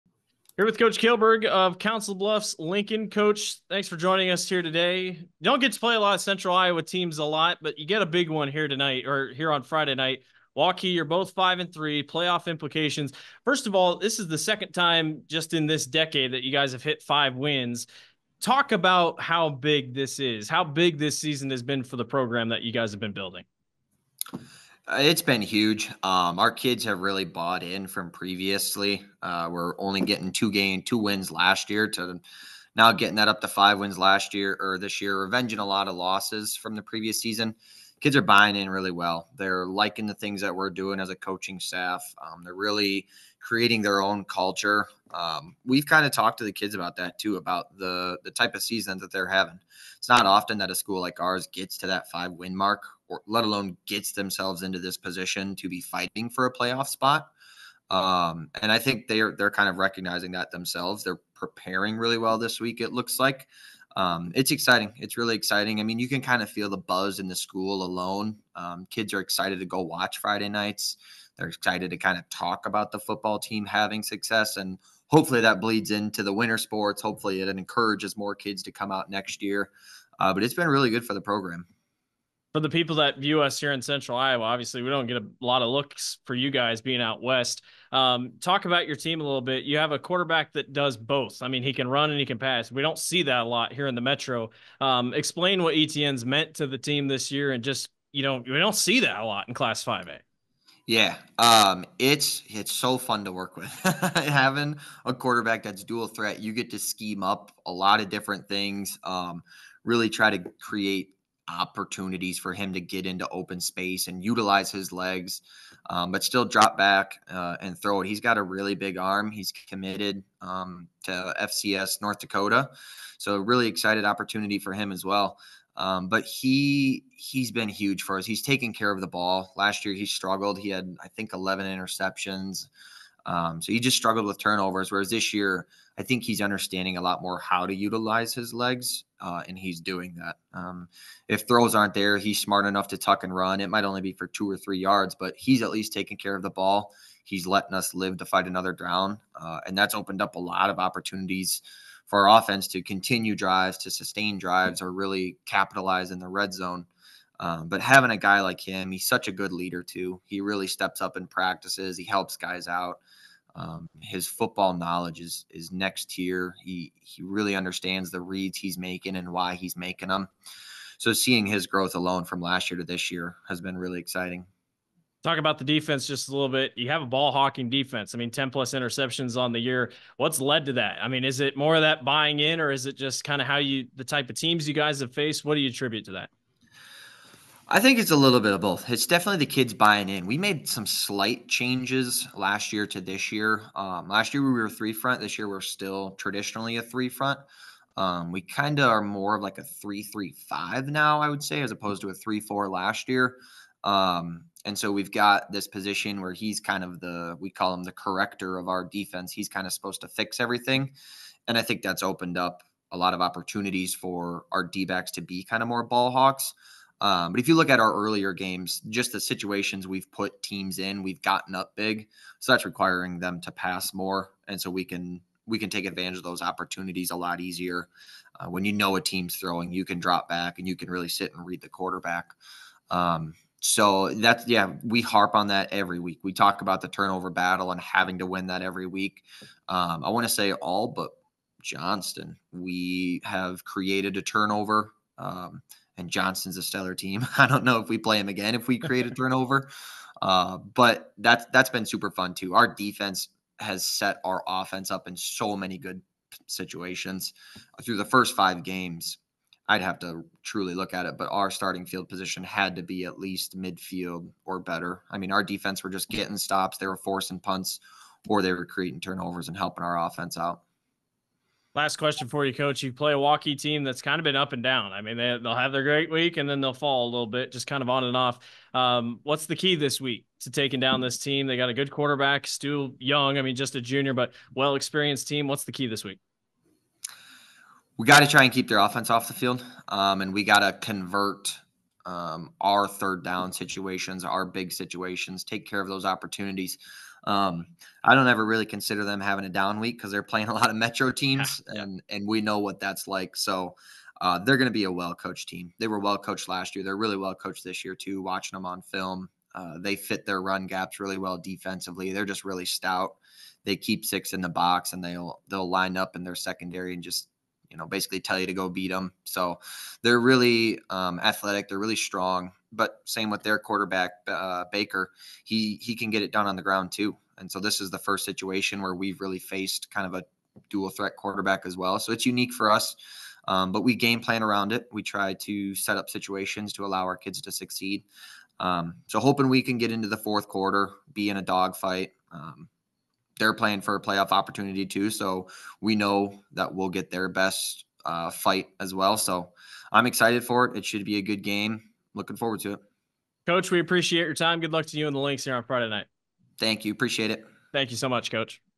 CISN Football Coach Interview: Council Bluffs Lincoln Head Coach Peter Kilburg ('24 Week 9)
Blake Walker chats with Council Bluffs Lincoln Head Coach Peter Kilburg about the Lynx' massive Week 9 showdown against Waukee.

Follow CISN on Twitter, Instagram, & Facebook down below for more coverage of Central Iowa High School Athletics & the Des Moines Menace!